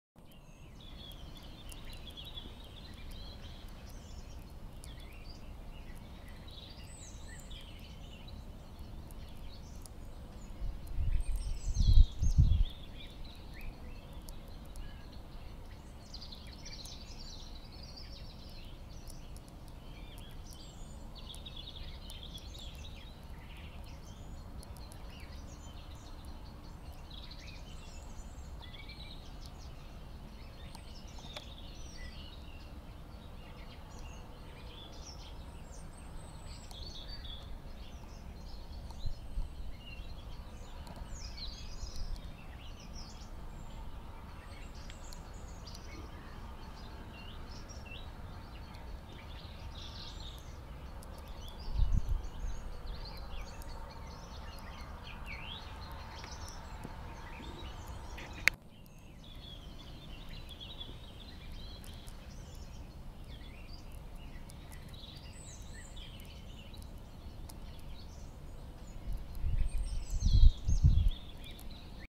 Mute swan swimming in a golden pond
En knölsvan simmar i en liten damm i södra Sverige vid solnedgången den 1 november 2014.